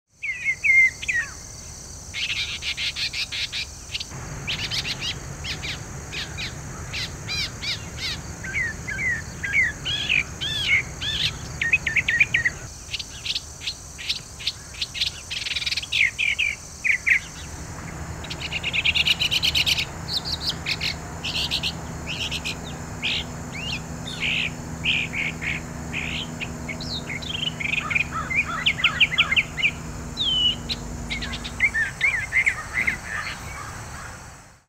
A NORTHERN MOCKINGBIRD SINGS
This Northern Mockingbird was putting on quite a performance, singing long and loud from high perches.  -  “Northern Mockingbirds typically sing from February through August, and again from September to early November. A male may have two distinct repertoires of songs: one for spring and another for fall.”